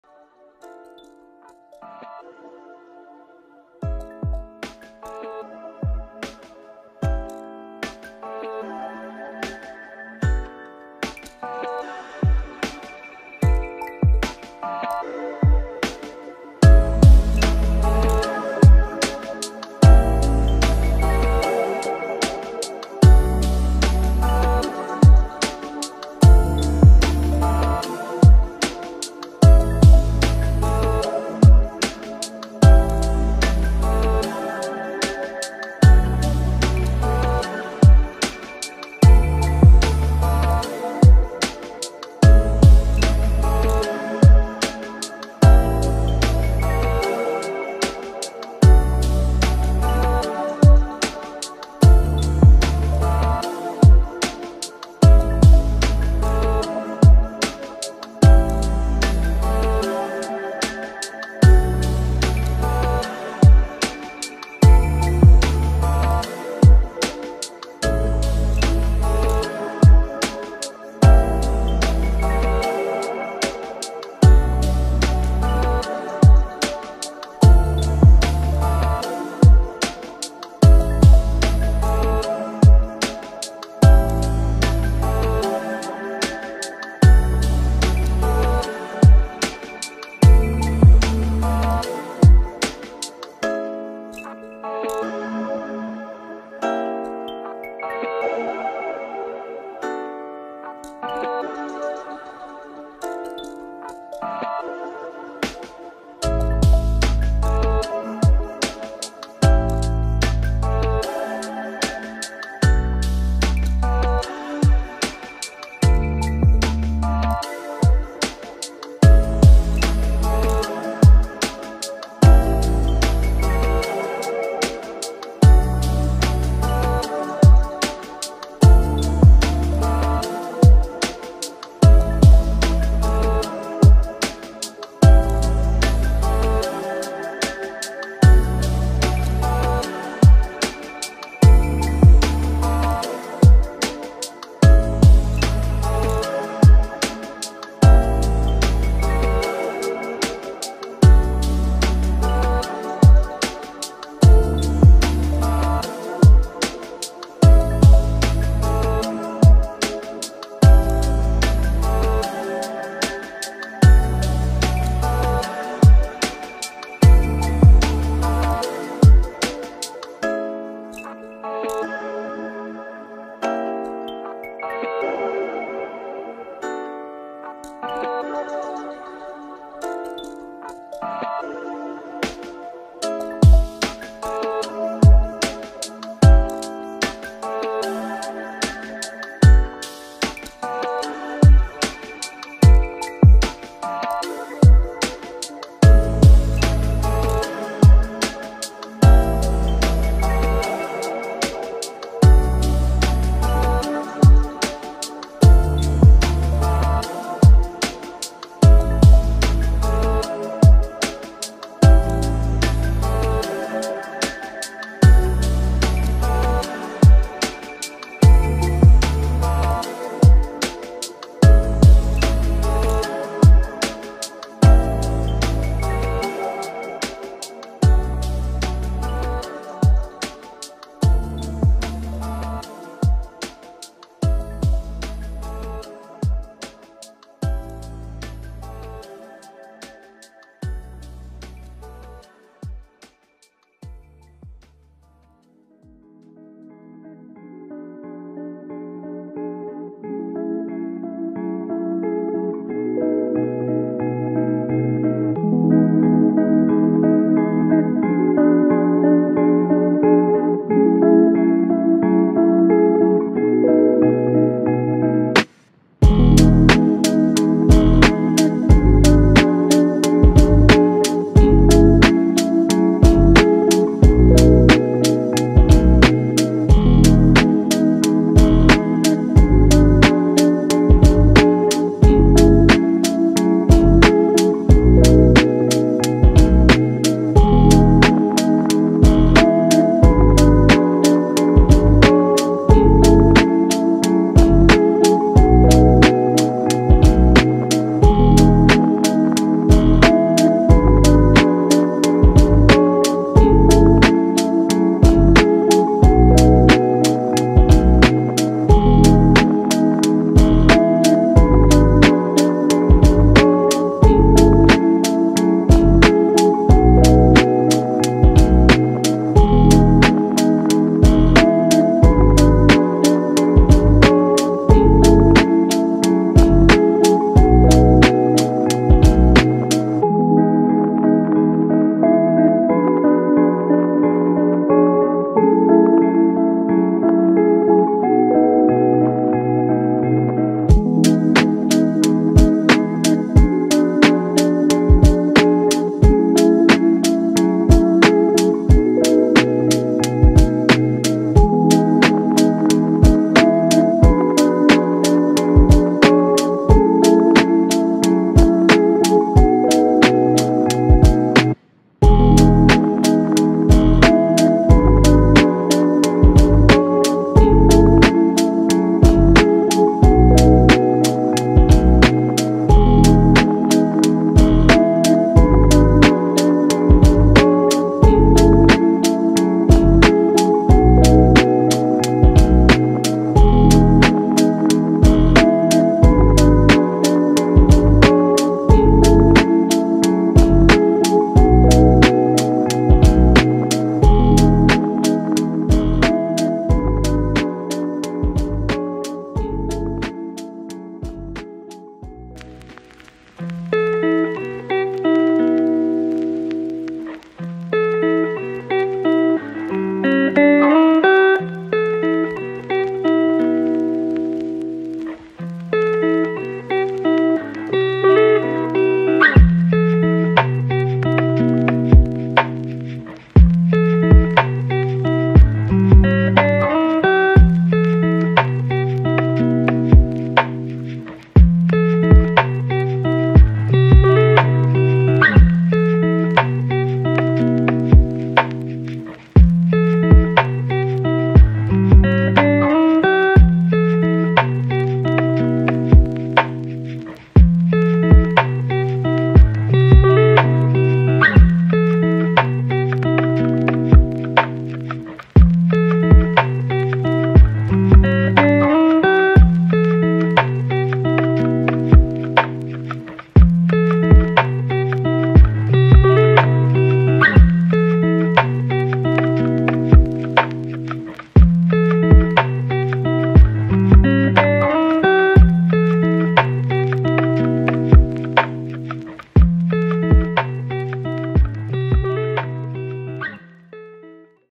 Bank of Korean Money Museum and Namdaemun Market | The Seoul Diaries Vlog #30
🤍 Vlog Music